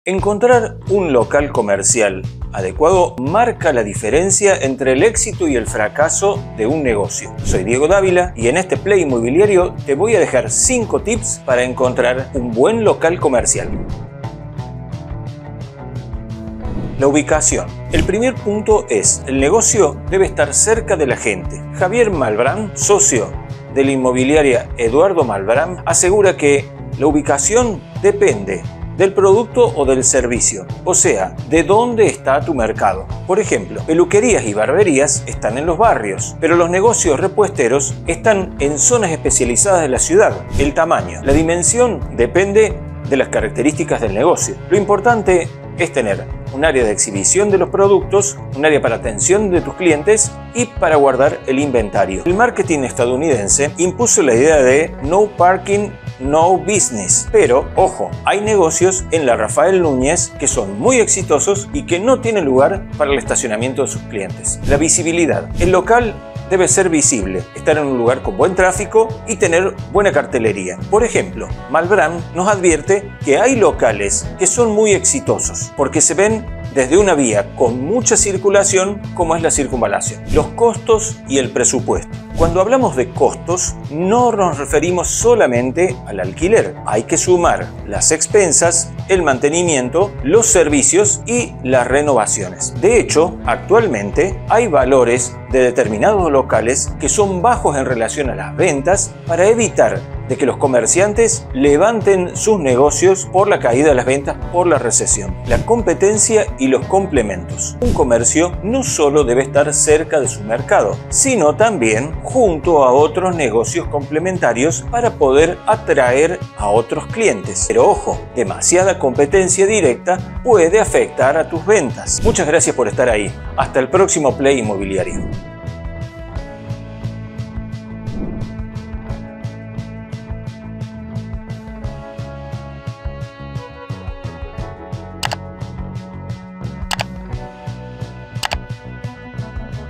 PLAY INMOBILIARIO: COMO ELEGIR EL MEJOR LOCAL COMERCIAL
Encontrar un local comercial adecuado marca la diferencia entre el éxito y el fracaso de un negocio. En Play Inmobiliario vamos a detallar cinco tips para elegir un buen local comercial.